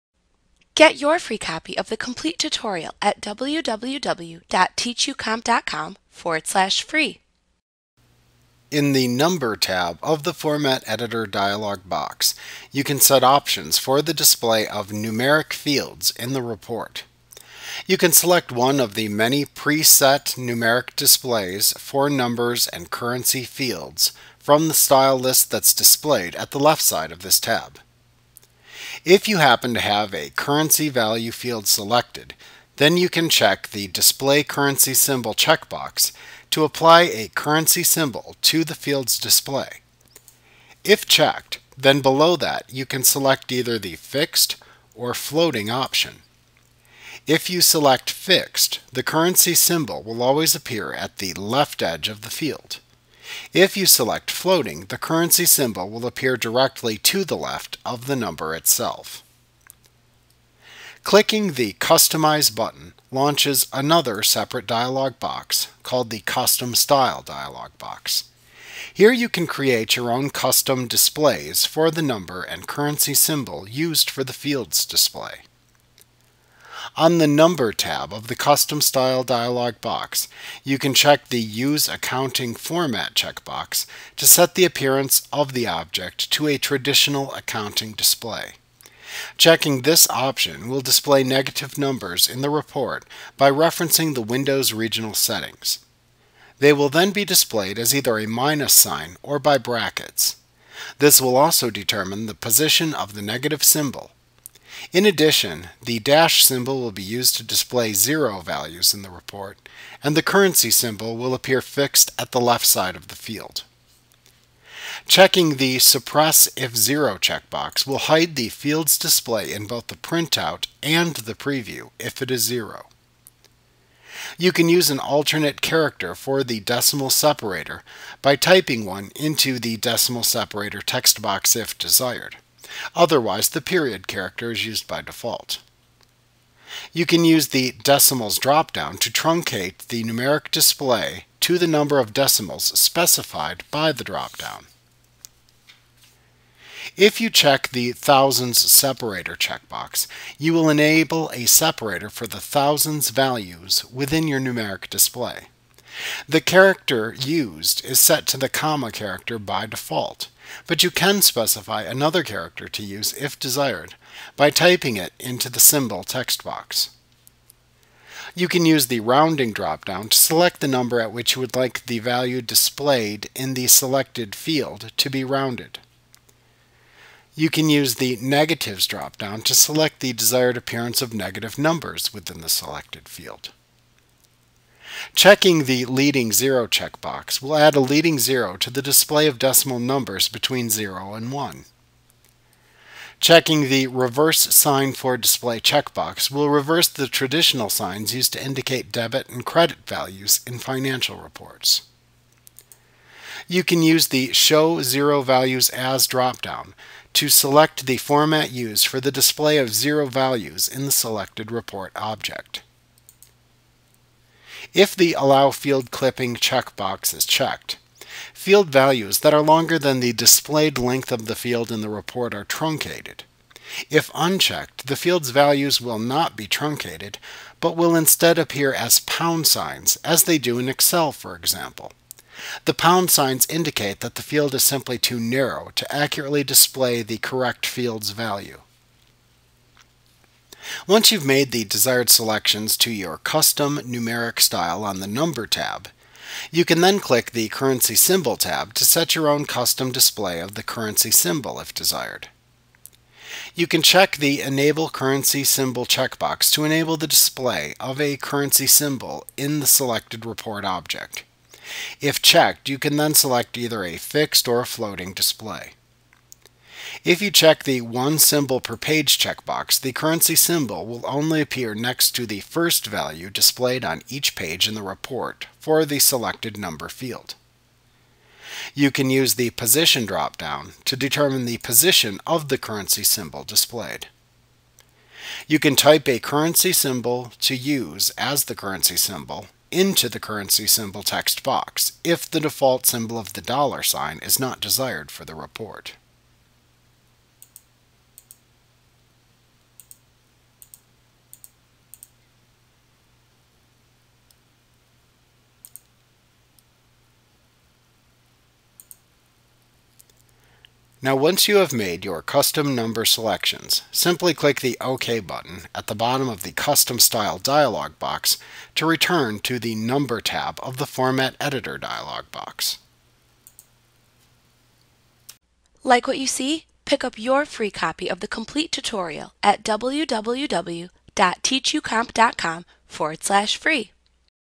Crystal Reports 2013 Tutorial The “Number” Tab of the Format Editor Business Objects Training
FREE Course! Learn about The “Number” Tab of the Format Editor in Crystal Reports by Business Objects A clip from Mastering Crystal Reports Made Easy v. 2013. - the most comprehensive Crystal Reports tutorial available. Visit us today!